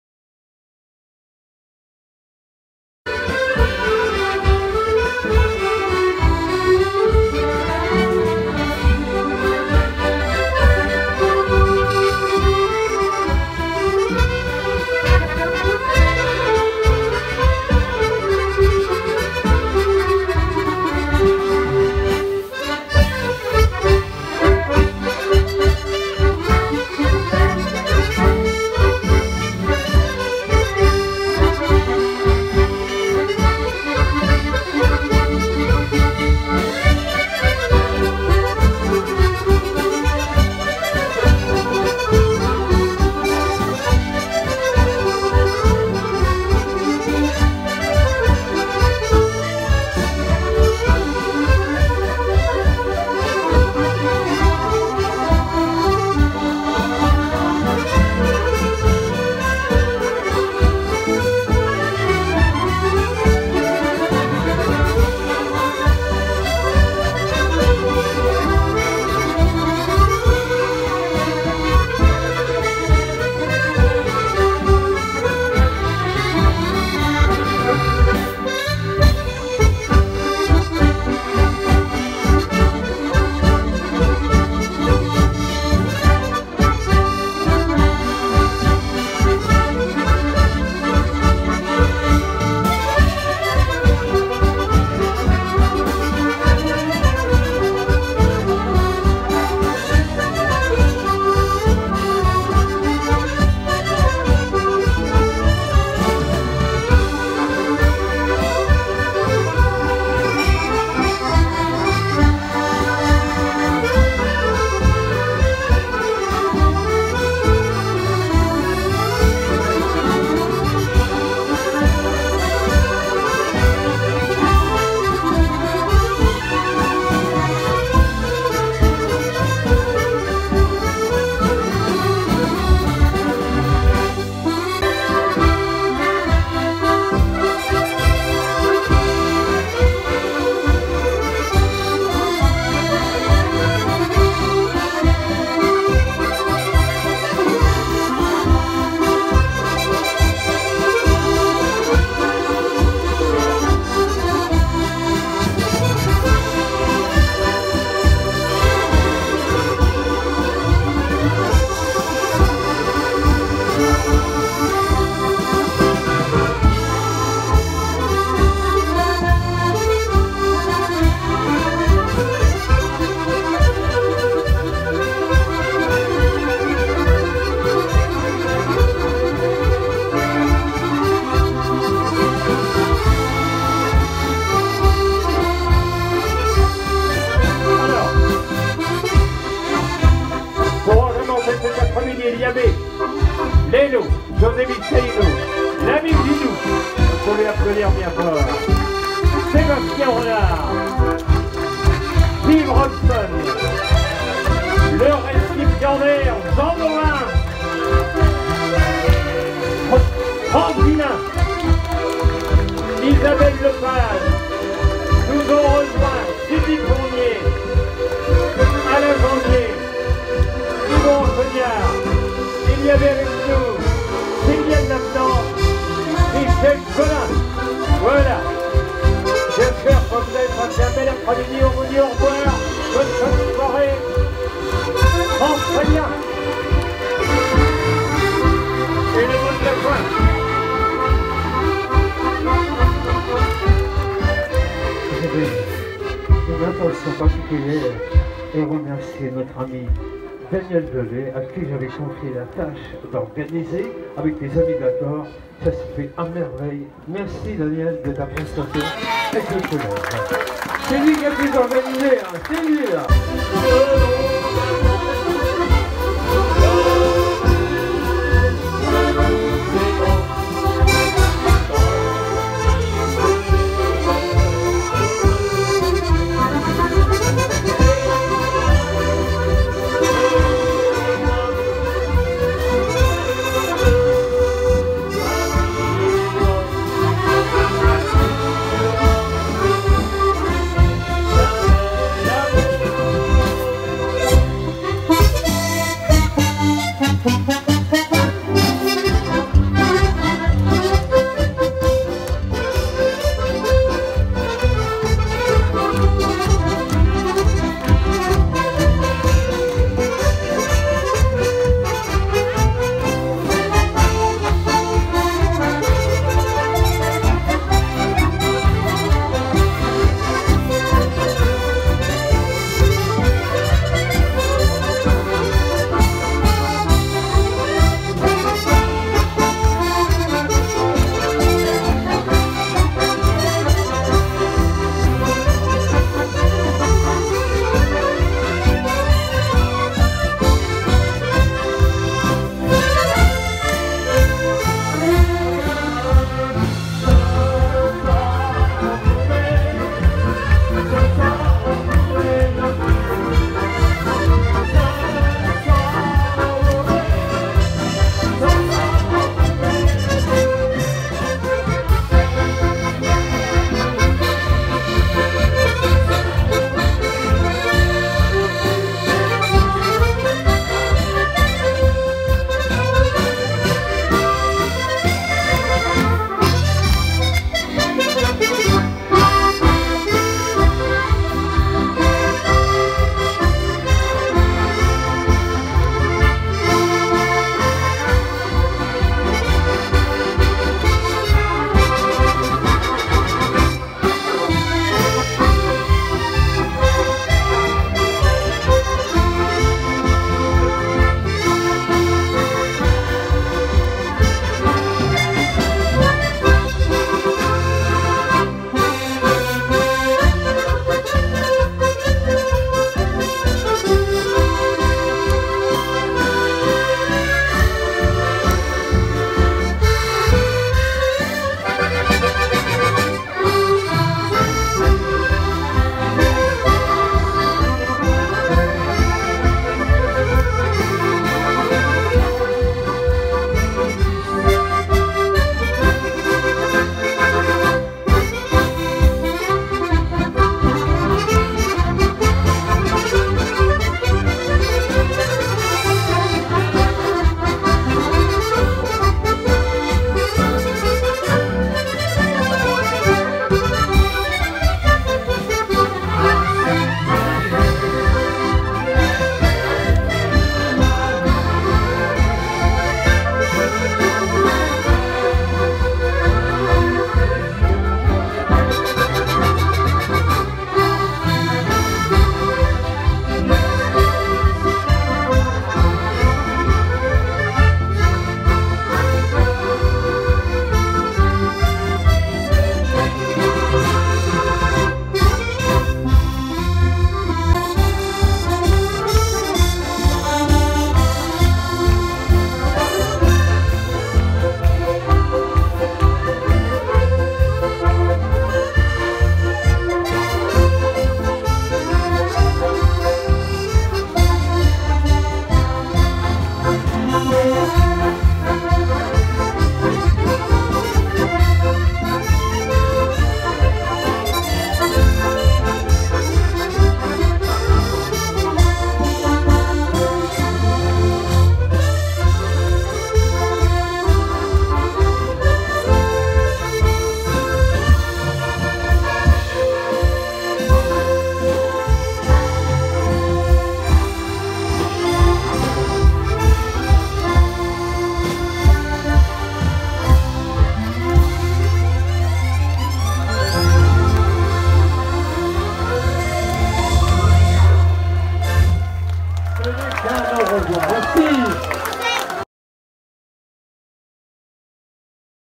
Jean DAUVIN “ 60 ans d ‘ accordéon “ final
Avranches (50) La salle Victor Hugo affiche complet pour les 60 ans d’ accordéon de JEAN DAUVIN …AVranchinais , au sein de la municipalité pendant des années . Merci aux maires d ‘ Avranches , St Senier certains élus pour leur présence et à tous les amis musiciens  venus nombreux  faire résonner leurs accordéons dans la baie du Mont St Michel ce 26 mars 2023 …..photos film Odile Retrodanse